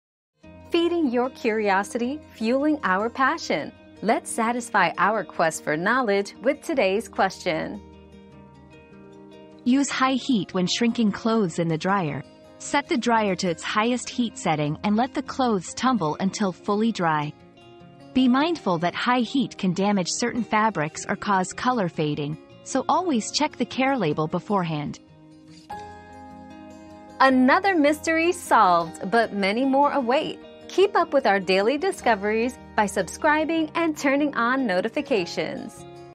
What temperature is best for shrinking clothes in the dryer?
Quick Tip: Shrinking Clothes in the Dryer 👉 Shrink Clothes Tips 👉 Learn the best temperature to shrink clothes in the dryer using high heat. Make sure to check the care label first to avoid damage or color fading.

Laura S. Harris (2024, May 18.) What temperature is best for shrinking clothes in the dryer?
    🌐 AskAbout.video